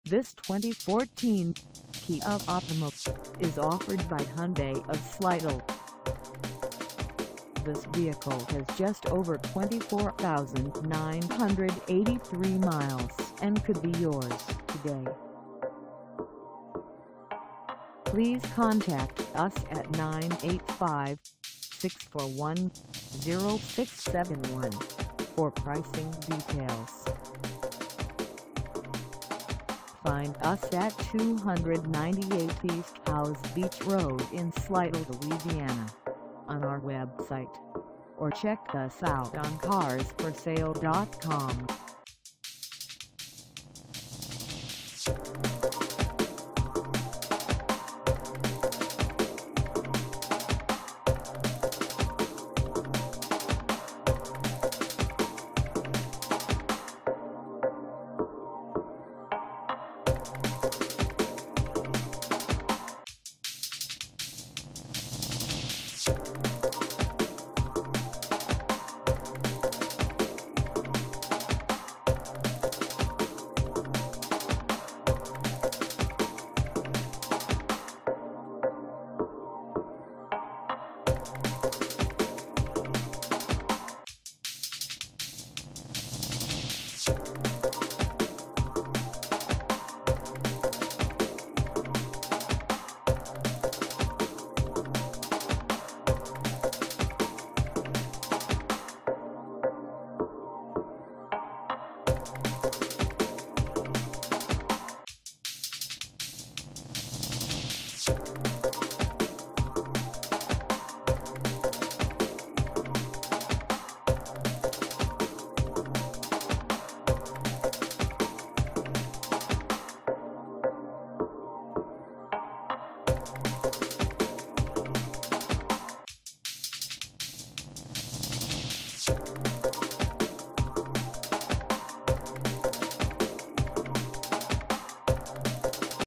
2014 Kia Optima for sale in Slidell, LA 70461 at the Hyundai
This 2014 Kia Optima is for sale in Slidell, LA 70461 at Hyundai Of Slidell.
Engine: I4 2.4L I4
Transmission: Automatic 6-Speed
Color: Snow White Pearl
Mileage: 24983
Contact Hyundai Of Slidell 298 East Howze Beach Road Slidell, LA 70461 to test drive this 2014 Kia Optima  today.